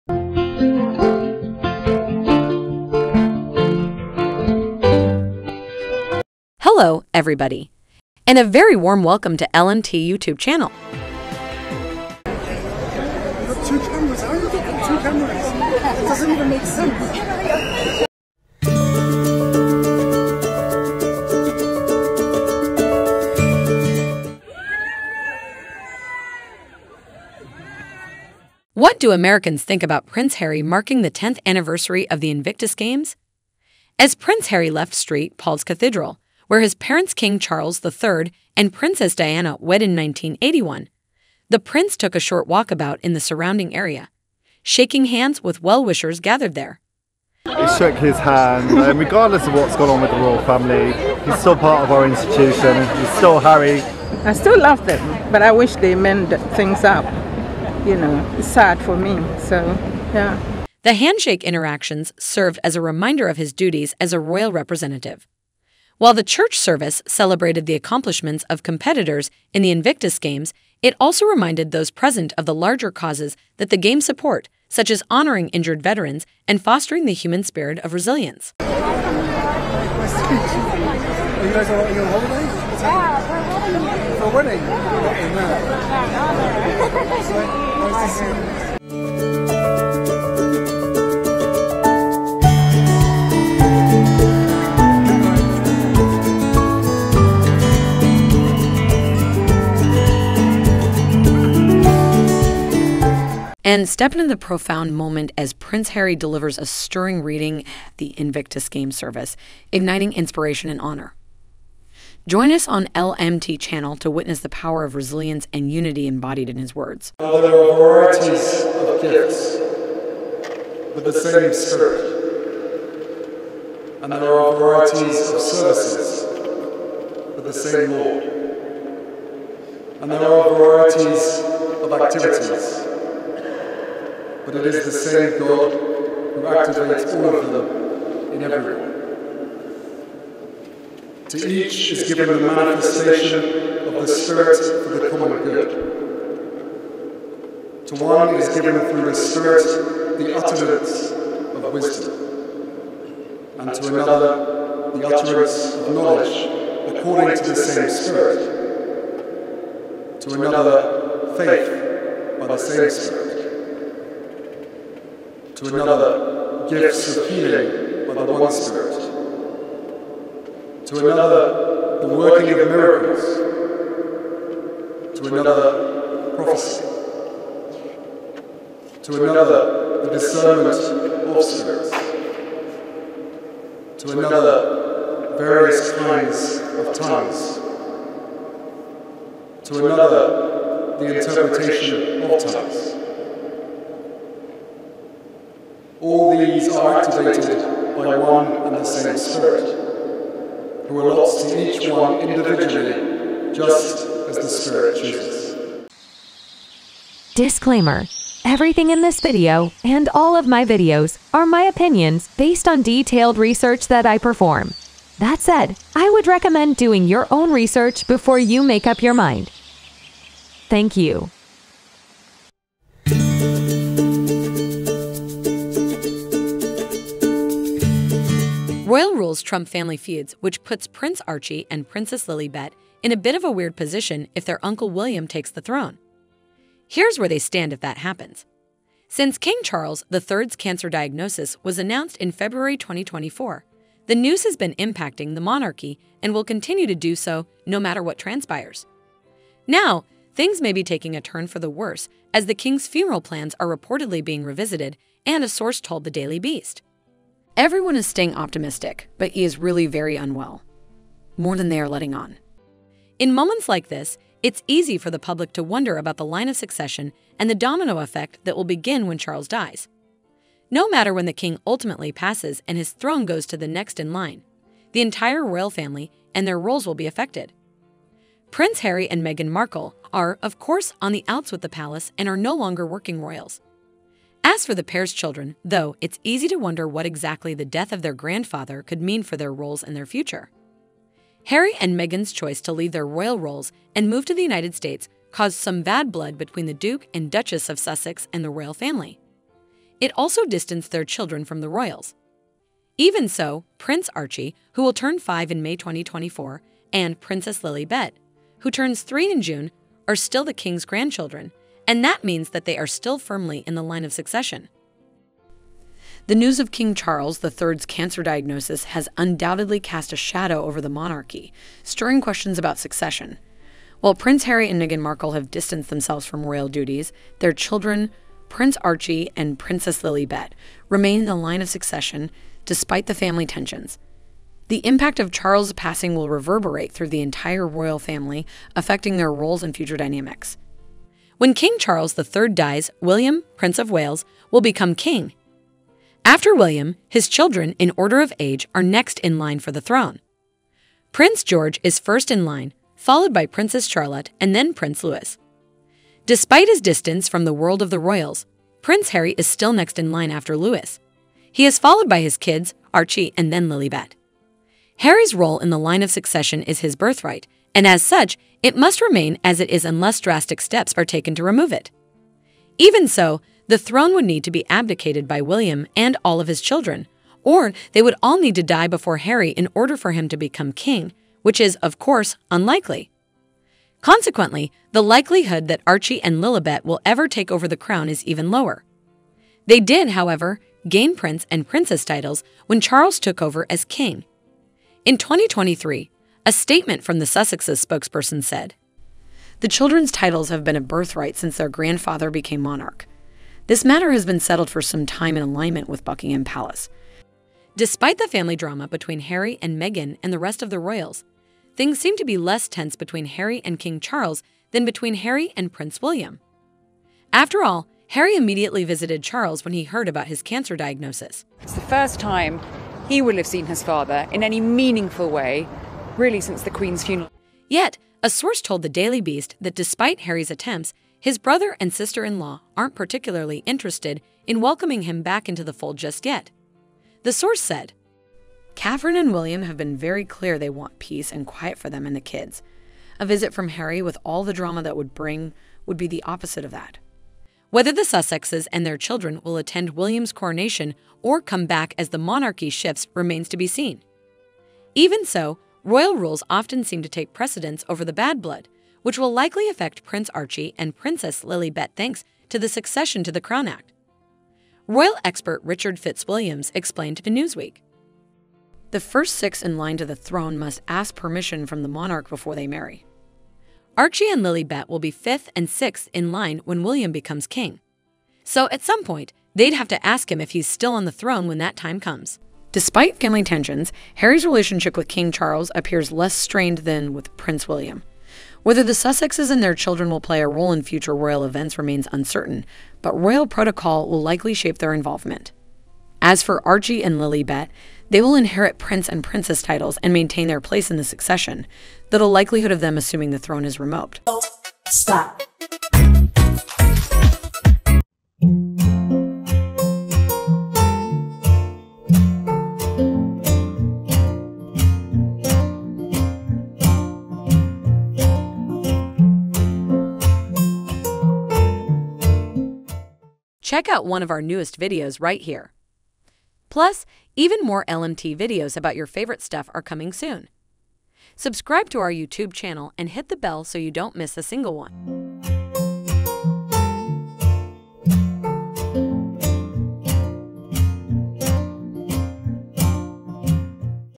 What do Americans think about Prince Harry now ?
What do Americans think about Prince Harry, The Duke of Sussex marking the 10th anniversary of the Invictus Games?

⤵ VIDEO CONTENT ⤵
Prince Harry - A ‘royal’ walkabout | 0:00
Archie & Lilibet: Future Royals Under King William | 3:42

Source: ITN, BBC, Reuters, The SUN